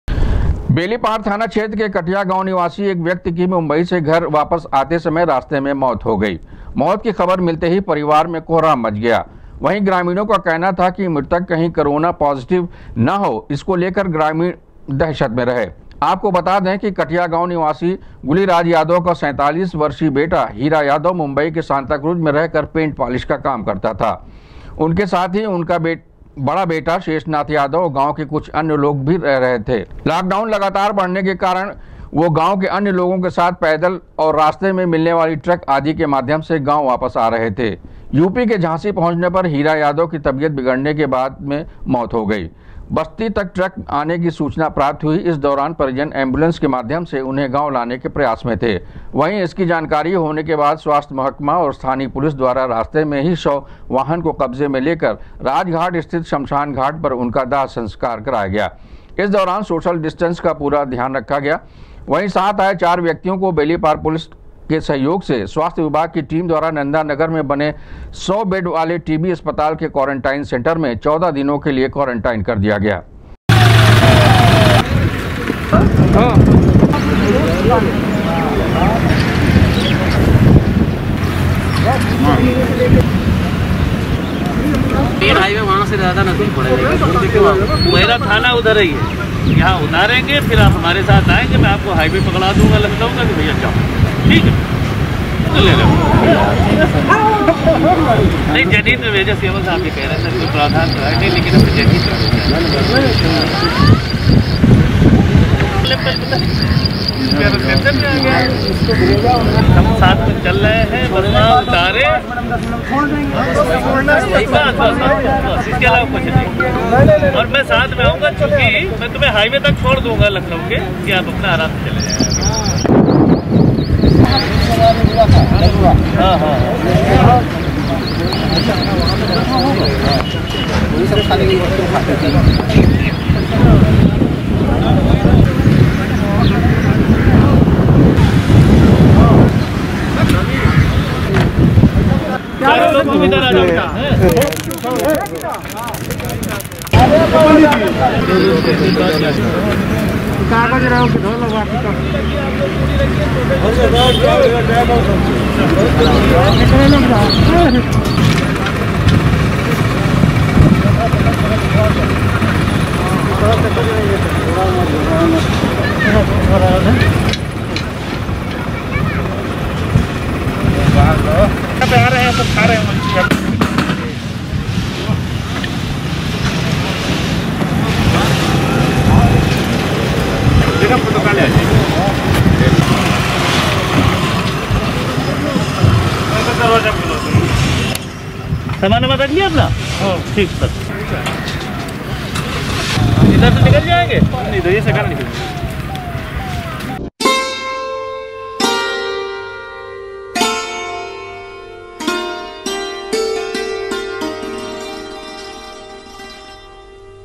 कटया गांव निवासी एक व्यक्ति की मुंबई से घर वापस आते समय रास्ते में हुई मौत, परिजन एंबुलेंस के
बेलीपार थाना क्षेत्र के कटया गांव निवासी एक व्यक्ति की मुंबई से घर वापस आते समय रास्ते में हुई मौत, परिजन एंबुलेंस के माध्यम से उन्हें गांव लाने का रहे थे प्रयास, जानकारी होने के बाद स्वास्थ्य महकमा और स्थानीय पुलिस द्वारा रास्ते में ही शव वाहन को कब्जे में लेकर राजघाट स्थित शमशान घाट पर कराया दाह संस्कार